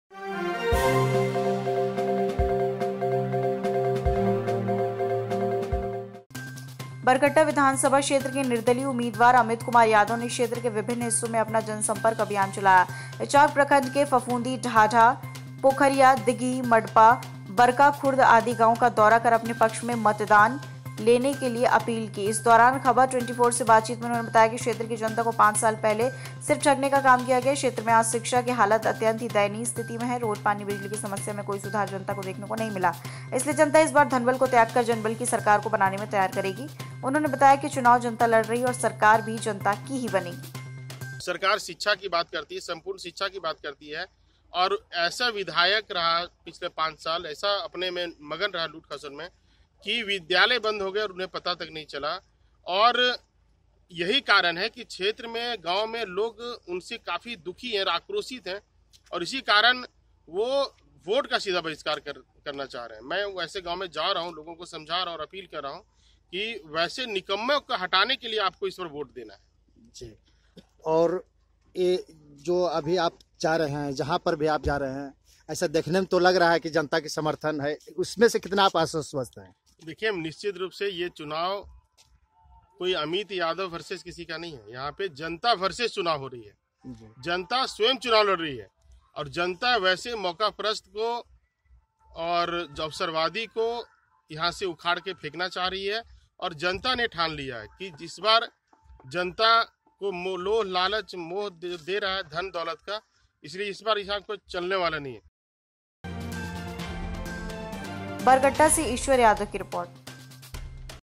बरकट्ठा:निर्दलीय उम्मीदवार अमित कुमार यादव ने क्षेत्र के विभिन्न हिस्सों में चलाया जनसंपर्क अभियान -
निर्दलीय उम्मीदवार अमित कुमार यादव ने क्षेत्र के विभिन्न हिस्सों में चलाया जनसंपर्क अभियान ---

बरकट्ठा विधानसभा क्षेत्र के निर्दलीय उम्मीदवार अमित कुमार यादव ने क्षेत्र के विभिन्न हिस्सों में अपना जनसंपर्क अभियान चलाया। इचाक प्रखंड के फफुंदी,डाढा,पोखरीया,दिगही, मडपा बरकाखुर्द आदि गांवों का दौरा कर अपने पक्ष में मतदान करने की अपील की। इस दौरान खबर 24 से बातचीत में उन्होंने बताया की क्षेत्र की जनता को पांच साल में सिर्फ ठगने का काम किया गया है। क्षेत्र में आज शिक्षा की हालात अत्यंत ही दयनीय स्थिति में है। रोड,पानी,बिजली की समस्या में कोई सुधार जनता को देखने में नहीं मिला इसलिए जनता इस बार धनबल को त्यागकर जनबल की सरकार बनाने को तैयार है। उन्होंने बताया की चुनाव जनता लड़ रही है। और सरकार भी जनता की बनेगी।

बाइट -निर्दलीय प्रत्याशी अमित कुमार यादव

बरकट्ठा से ईश्वर यादव की रिपोर्ट